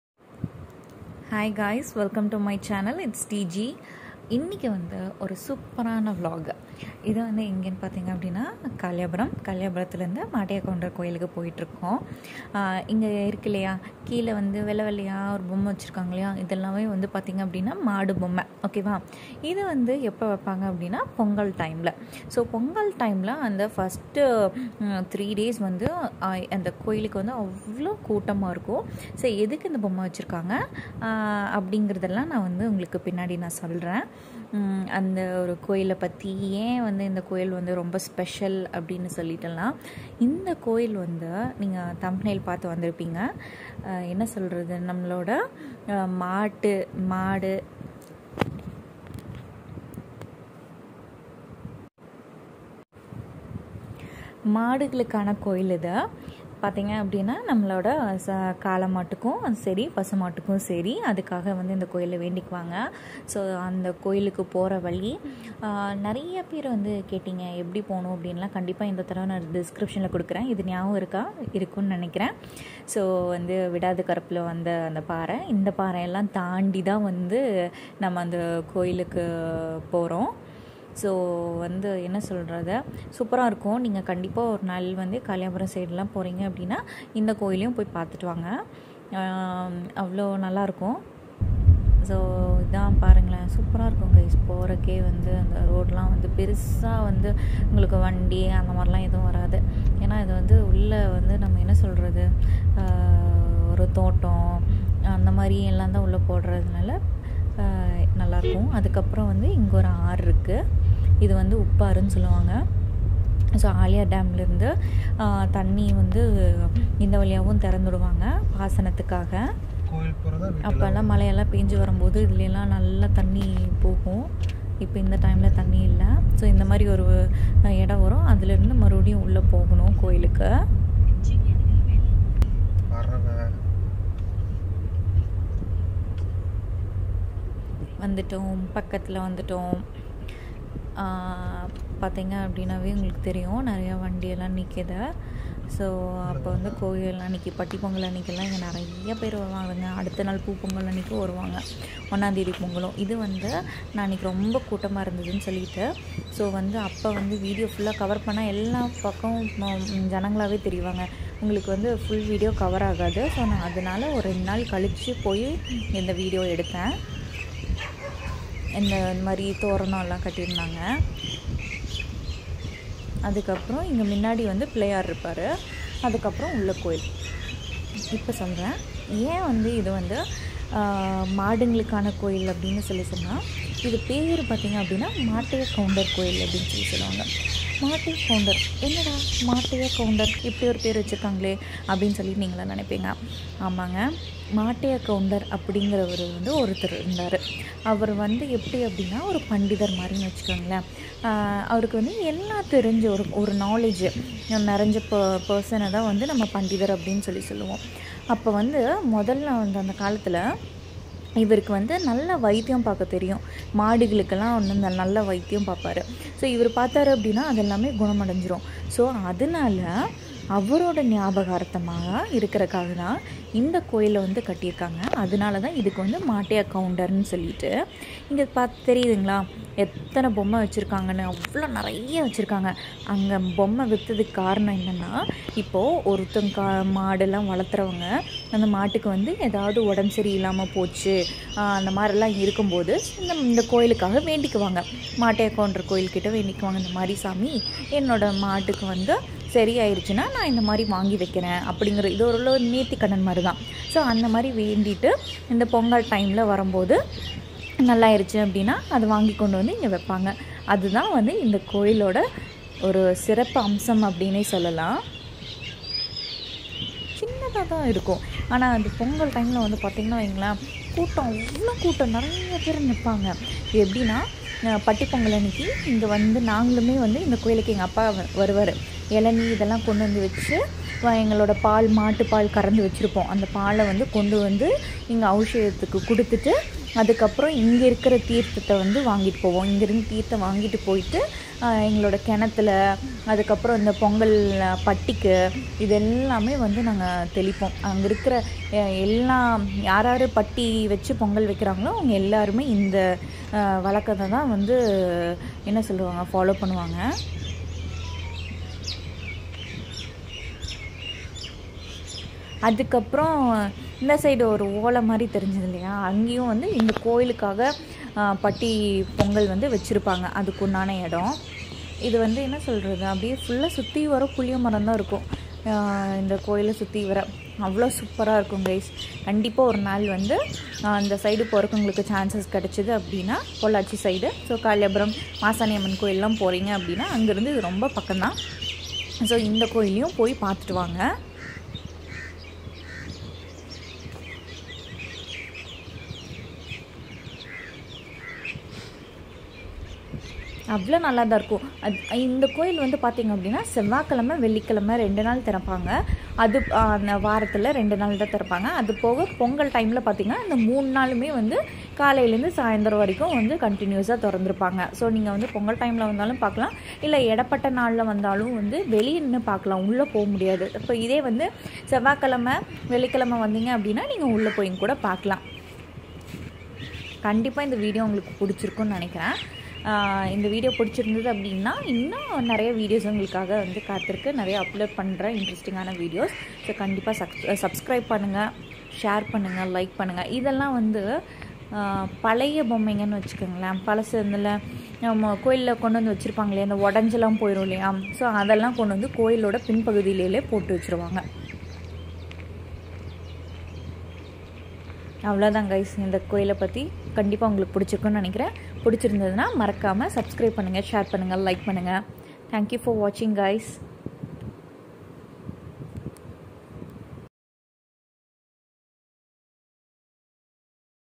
Temple for cattle | mattaekounder temple in kaliapuram | pollachi
This video is about a famous temple in our village for cattle, each and every farmer will worship here for well being of their cattle, festivals take place during pongal time for the first three days.